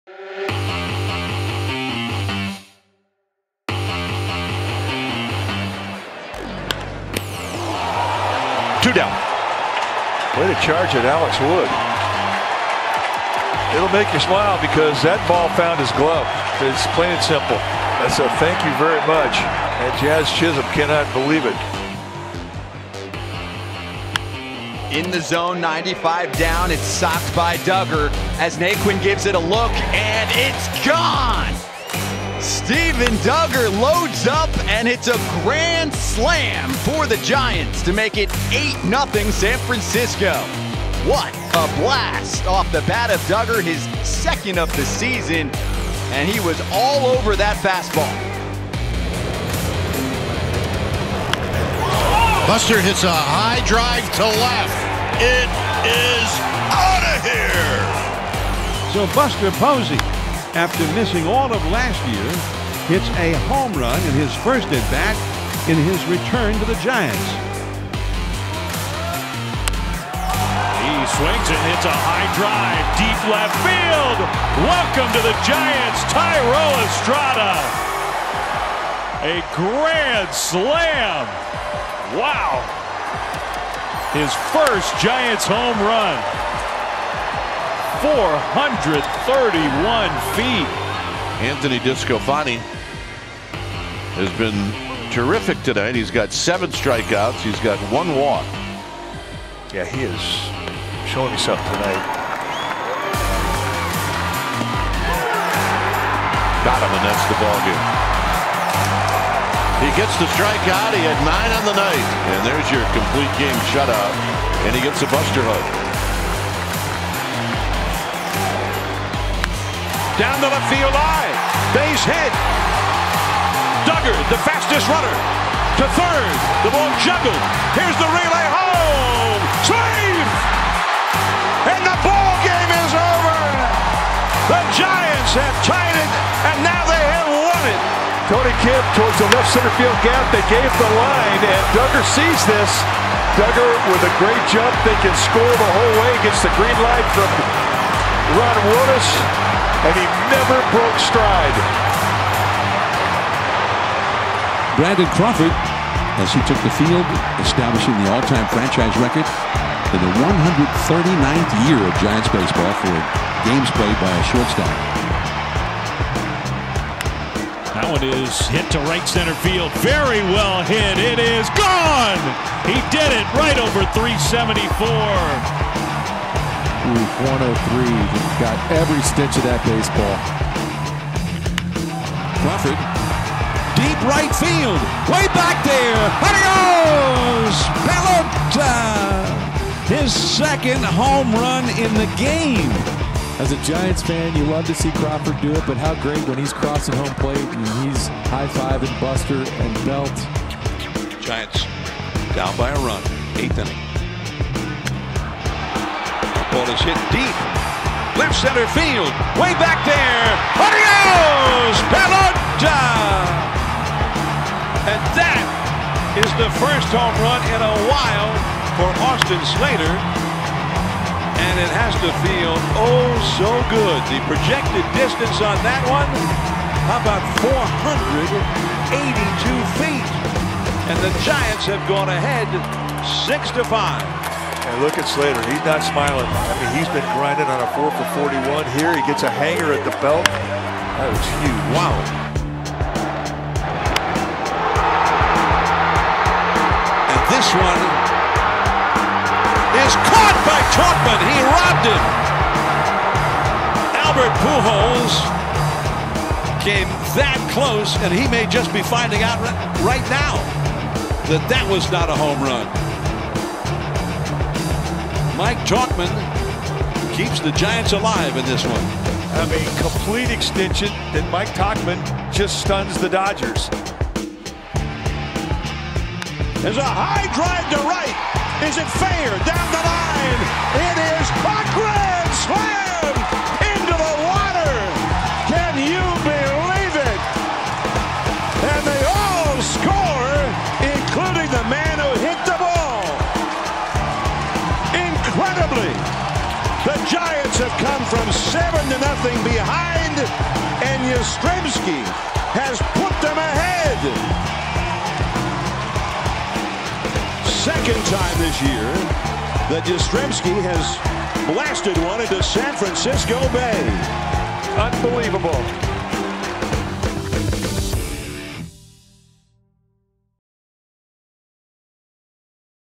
2021: Top 10 Plays of the First Half
The Giants entered the 2021 All-Star break with the best record in baseball. Let's look back on our Top 10 plays of the first half.

Do you agree? Let us know your Top 10 in the comments below.